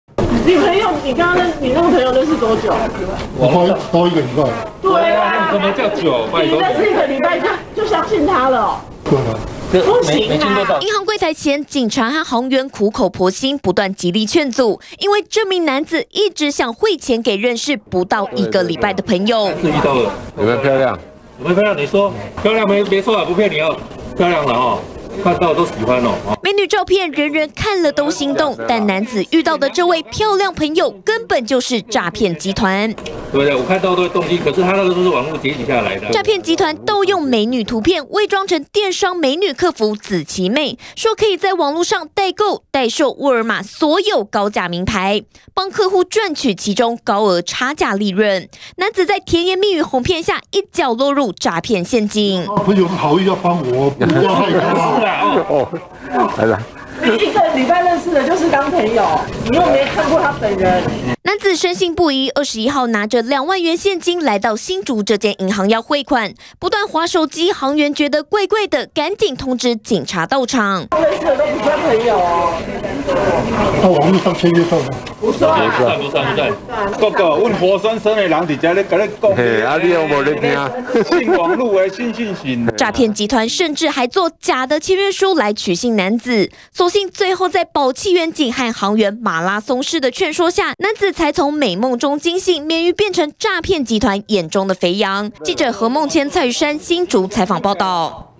20201026中天新聞 美女客服「紫琪妹」揪賺錢 男子險遭網路詐騙
民眾遭到網路詐騙的案例層出不窮，這次詐騙集團又出招，盜用美女圖片，偽裝成電商美女客服「紫琪妹」，聲稱可以代購、代售，知名大賣場的所有的高價名牌，幫被害人賺取高額差價利潤，男子在甜言蜜語哄騙下，帶著現金要到銀行匯款，眼尖行員察覺有異，通知員警到場，在大家極勸說下，男子才恍然大悟，自己被詐騙集團盯上了。